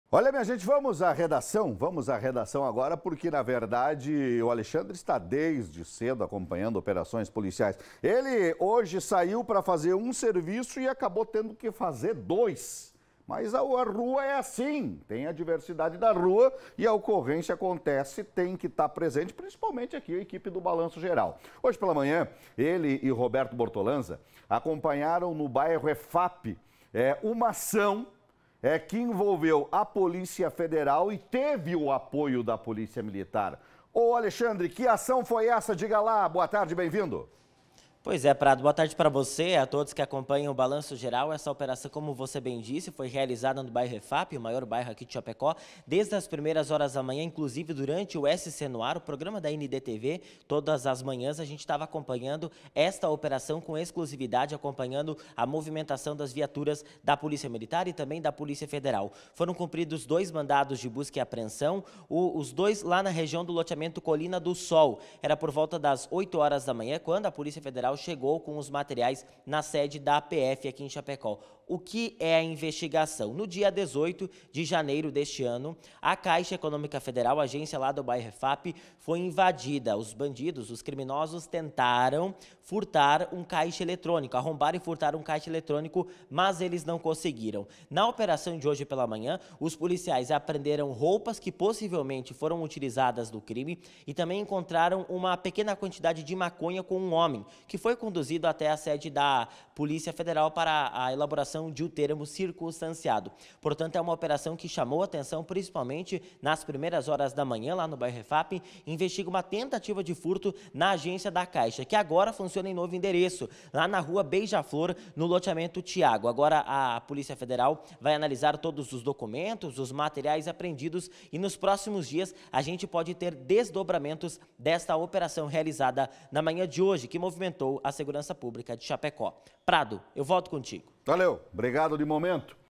Operação Policial em Chapecó: Tentativa de Furto em Agência Bancária é Alvo de Investigação
Uma operação da Polícia Federal está em curso em Chapecó, investigando uma tentativa de furto em uma agência bancária localizada no bairro Efapi. As autoridades estão empenhadas em esclarecer os detalhes desse incidente e garantir a segurança da região.